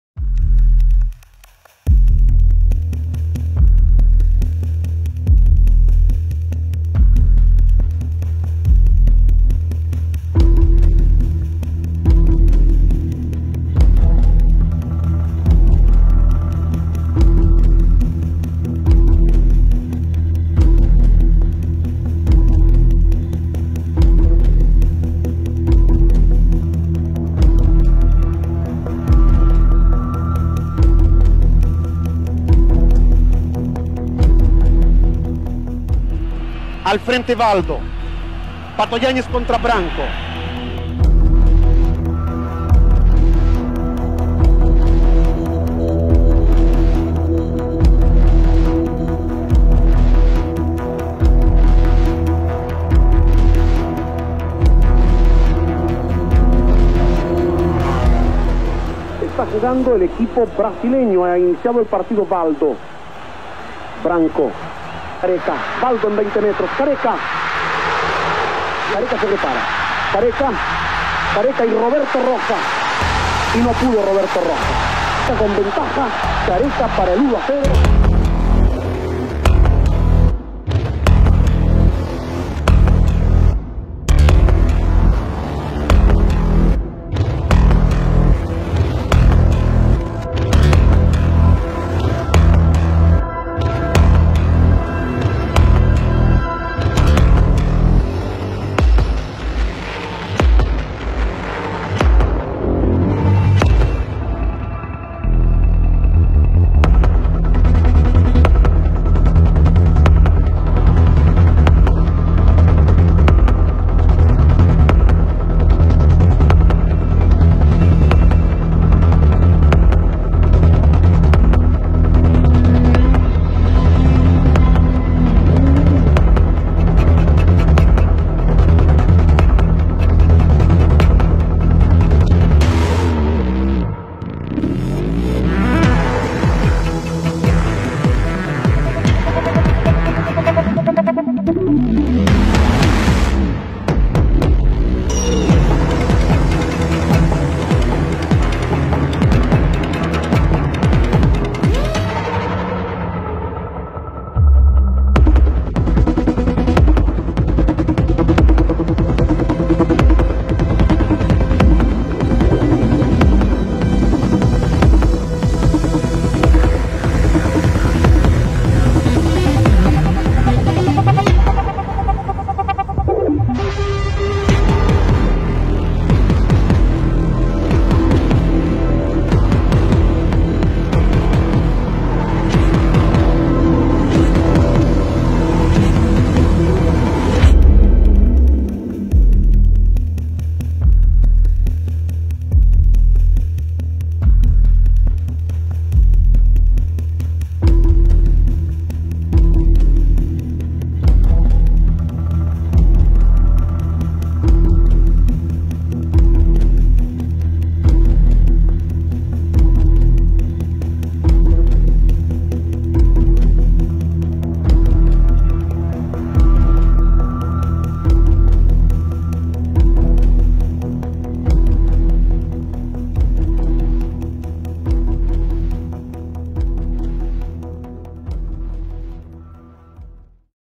O dia em que o Chile participou em uma das maiores vergonhas do futebol mundial
No ano 1989 o Chile devia jogar contra o Brasil no Maracanã para definiar a classificação para a copa do munda da Itália 1990. No entanto, com o placar em contra, um dos líderas daquela seleção, Roberto “Cóndor” Rojas, estrelou um nos momentos mais vergonhosos da história do futebol sul-americano e mundial.
Nos dias posteriores, o “Cóndor” apareceria tentando explicar o acontecido. No entanto, as suas declarações não fariam mais do que deixar um monte de dúvidas, que acabariam bruscamente com a sua carreira.